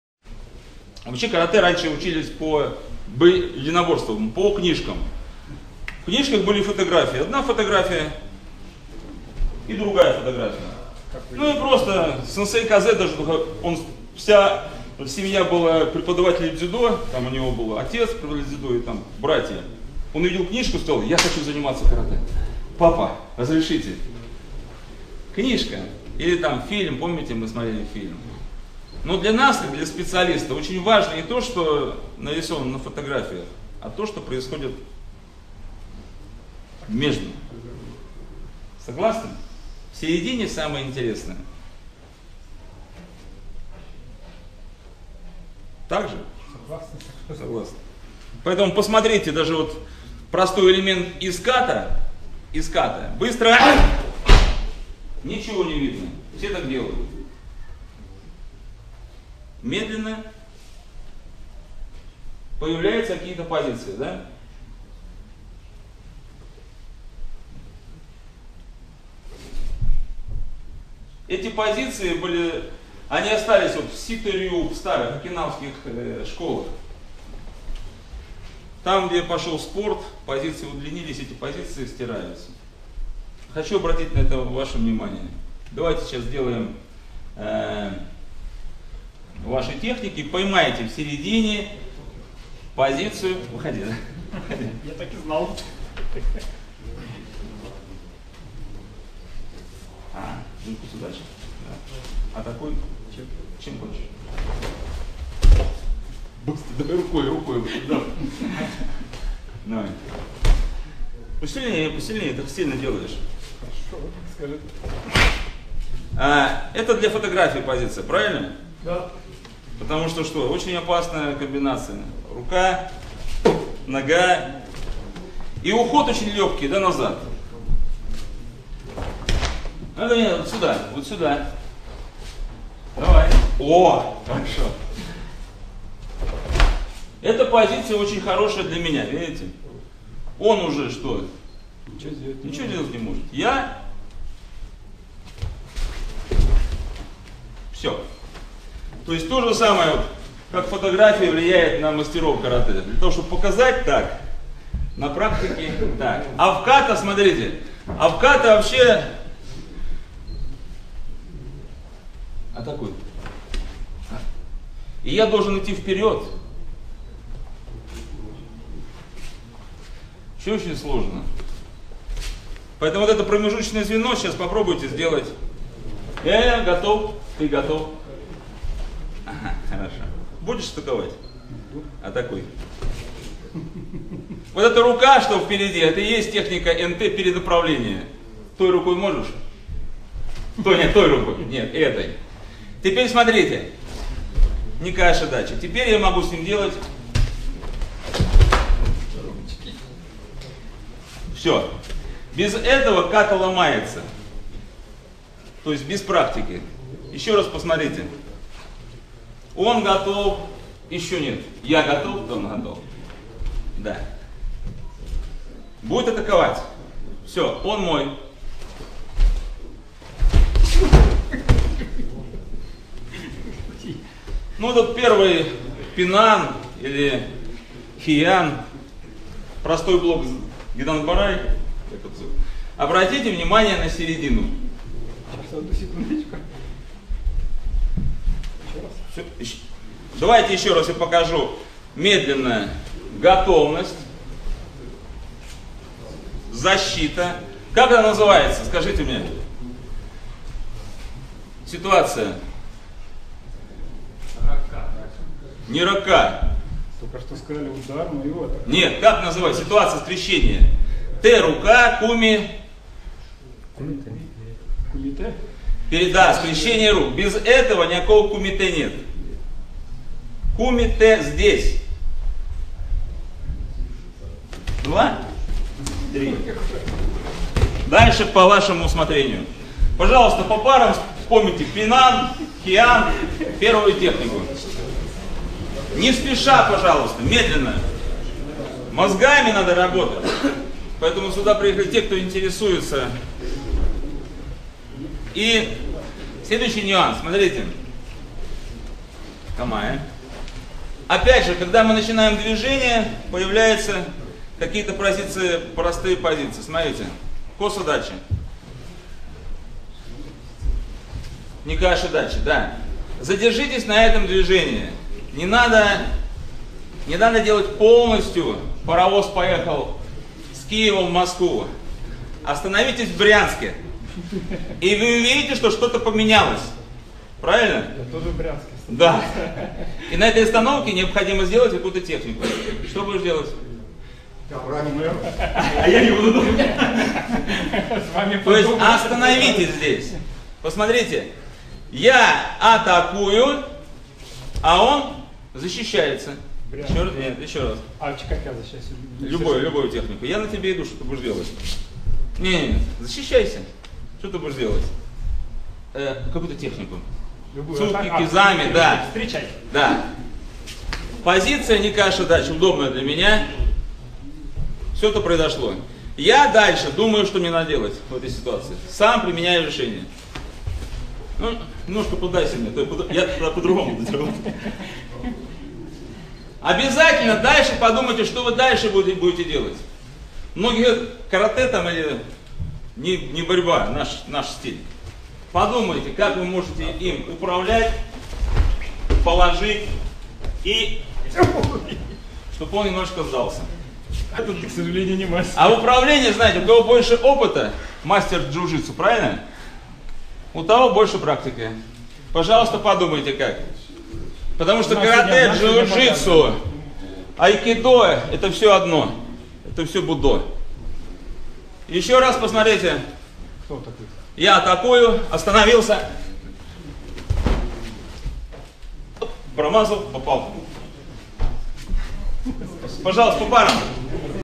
Айкидо в Каратэ - Невидимые приёмы
Это клип из ролика
" Читаем ката_ перемещения. Игорь Садовников".